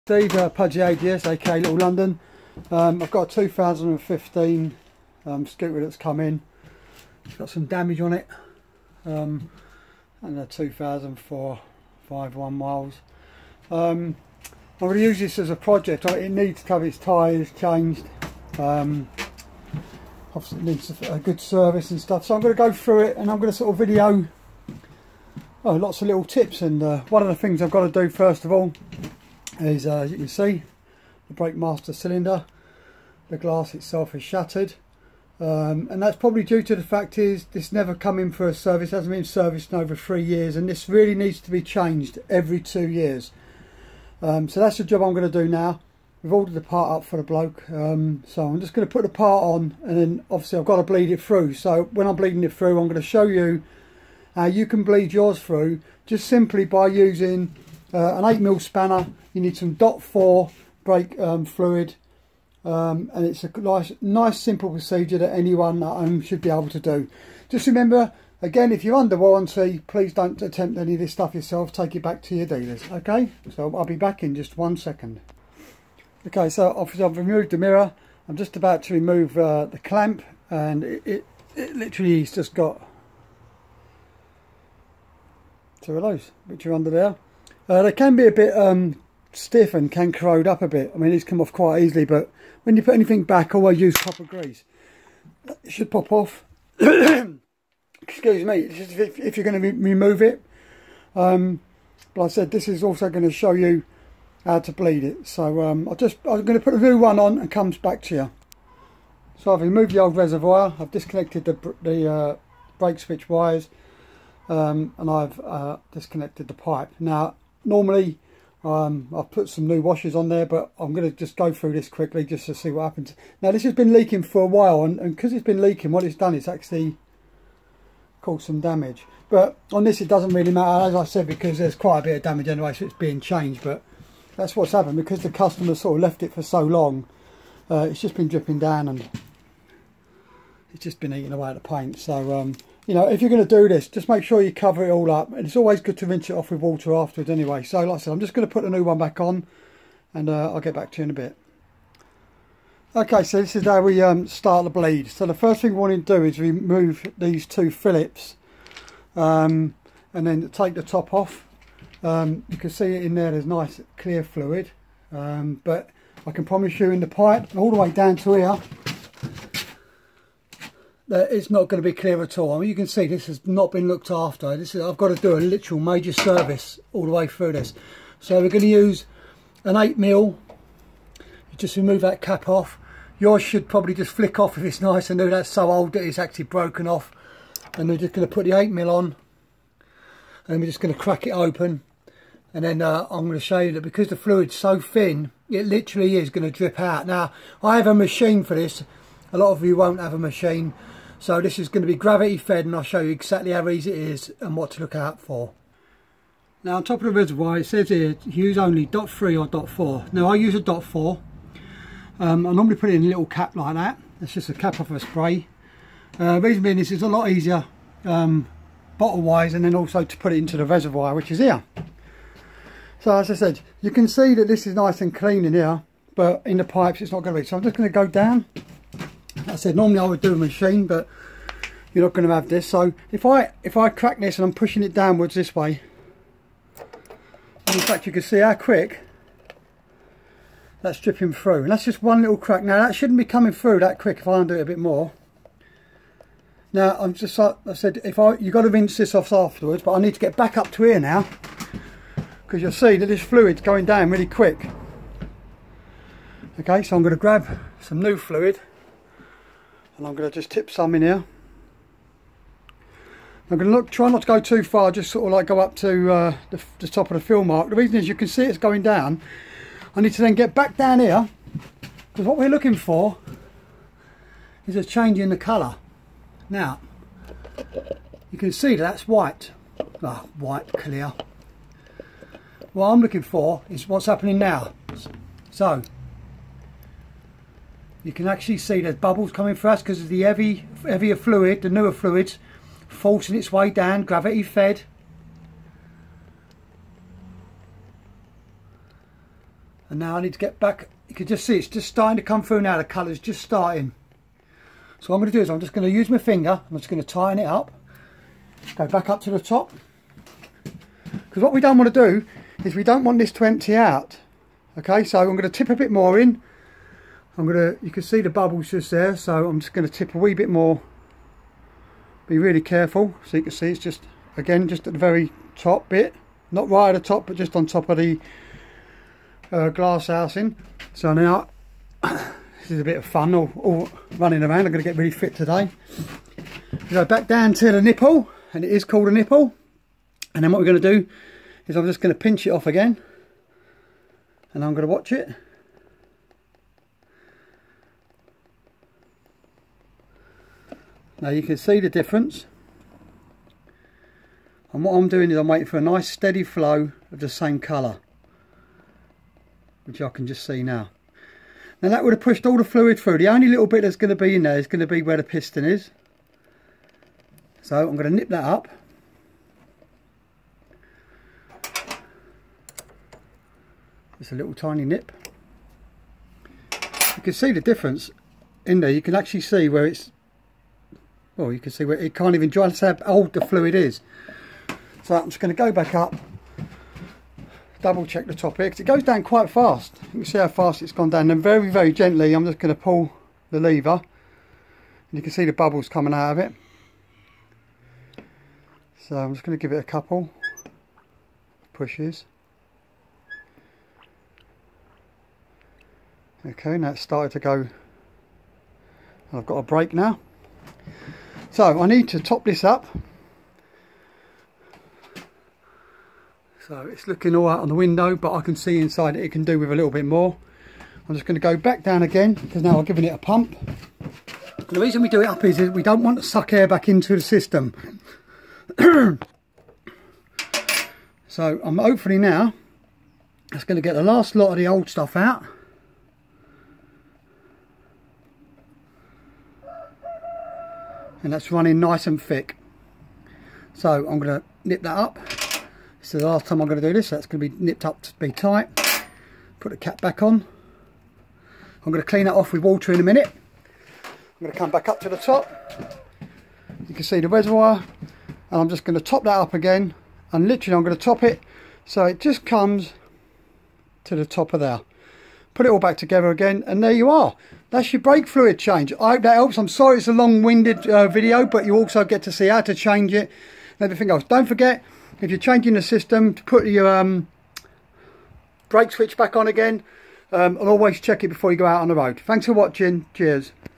AJS Modena 125 and 50cc brake fluid
Front Brake bleeding and reservoir replacment.
Always read the Labels. If your scooter is still under warranty please take it back to the dealer. I'm not responsible for any damage cause.